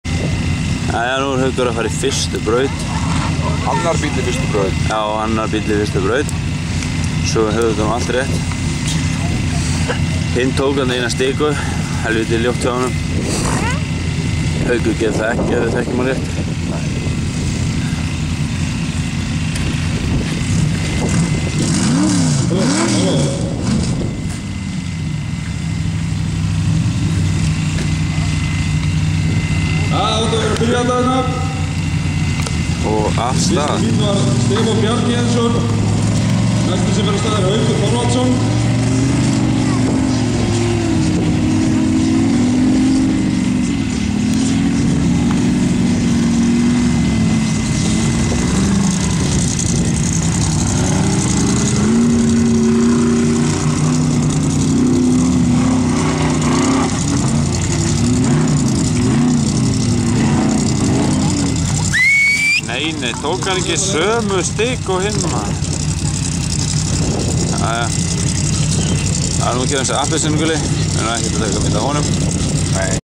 Offroad Iceland
Haukur í fyrstu braut á Egilstöðum 2009